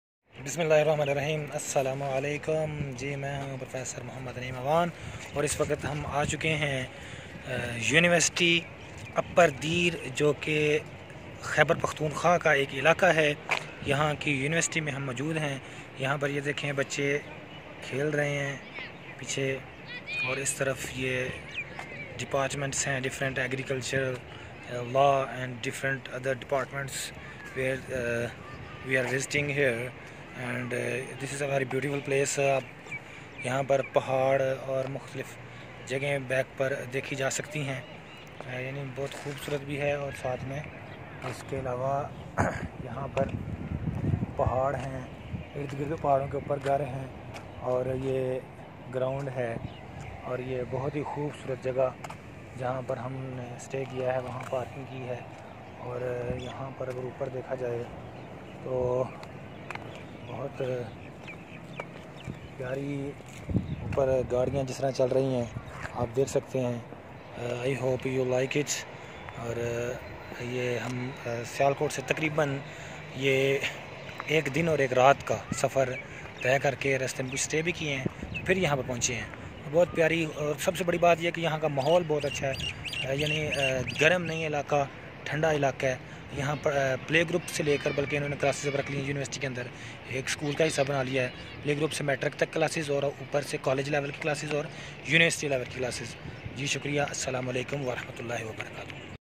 خیبر پختون خواہ دیر بالا میں بہت پیاری یونیورسٹی کی سیر اوار معلومات || By Prof. Naeem
Upper Dir a beautiful university visited by Prof. Naeem and his friends.